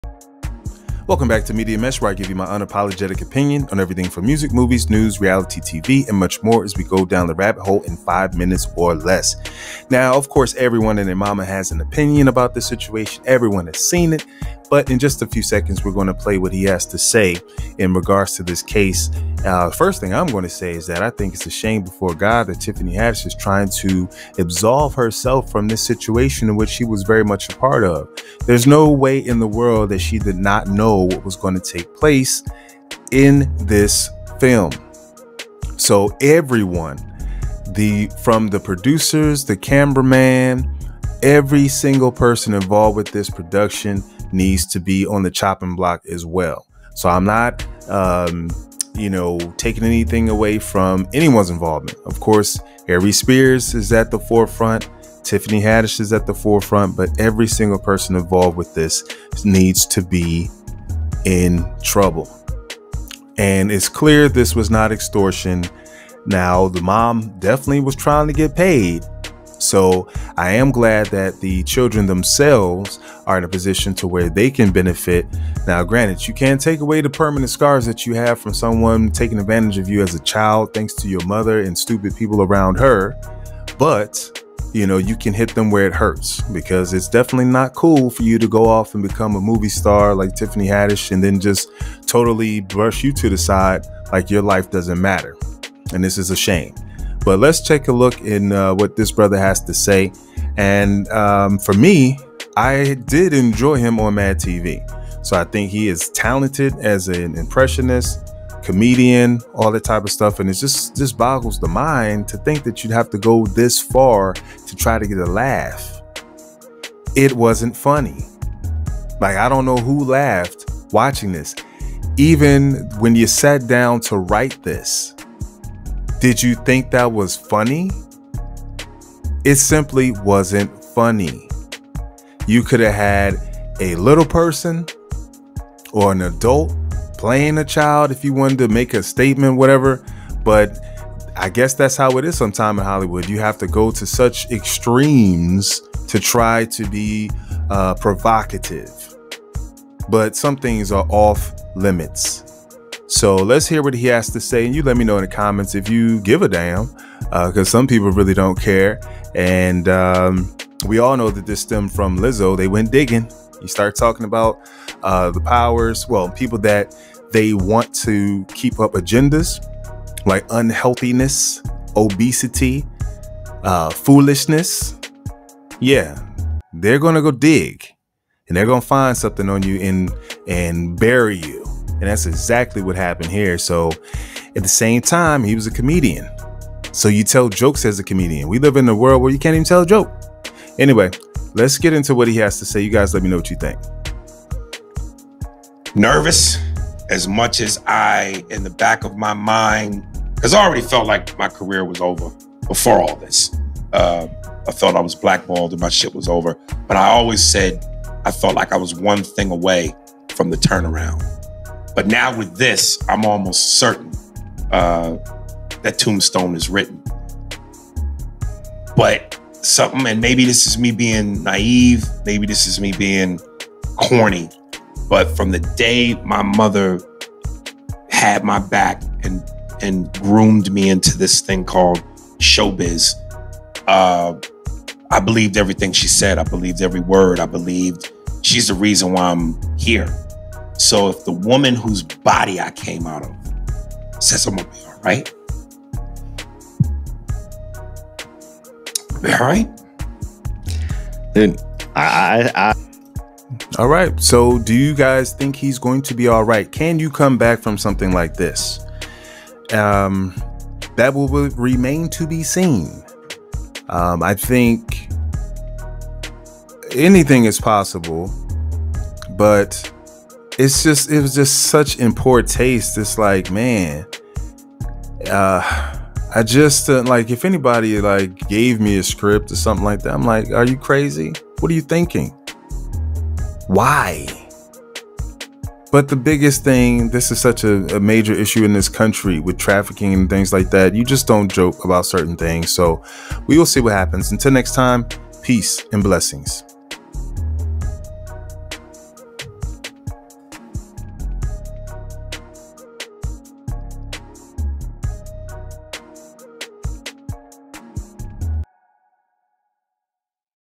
ARIES SPEARS AND TIFFANY HADDISH SETUP?
Aries Spears left fans scratching their heads recently after the comedian appeared to be reflecting on the current status of his career.

The “MadTV” star’s admission comes days after a woman, identified as Jane Doe asked Los Angeles District Attorney George Gascón in a written letter to prosecute and arrest Spears and Tiffany Haddish for allegedly “molesting” and “grooming” the 22-year-old and her brother when they were children. In addition to the written letter, Jane Doe filed a 30-page lawsuit with the Los Angeles Superior Court.